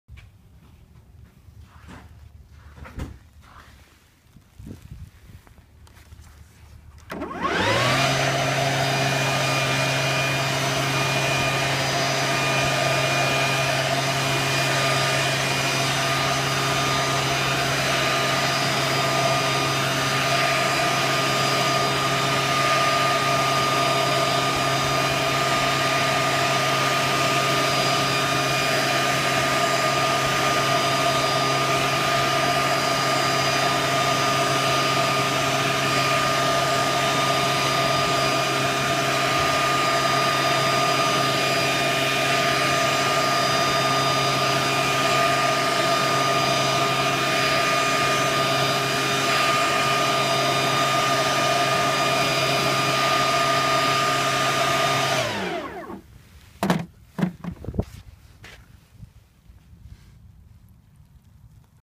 Stihl BGA 85 battery powered blower moving snow
Not bad for battery powered.